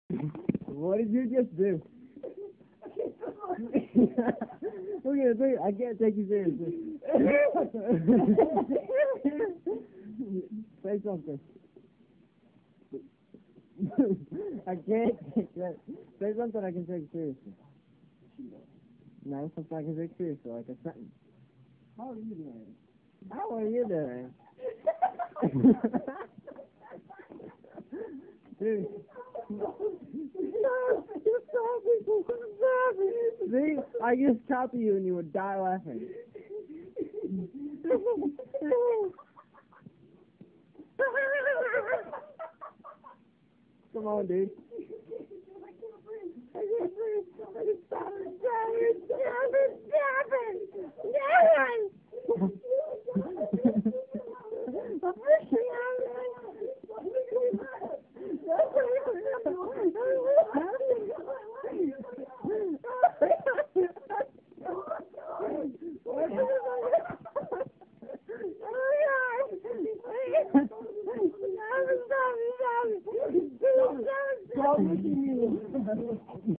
Josh mccrunk cant stop laughing
He couldn't catch his breath lol friend can't making him laugh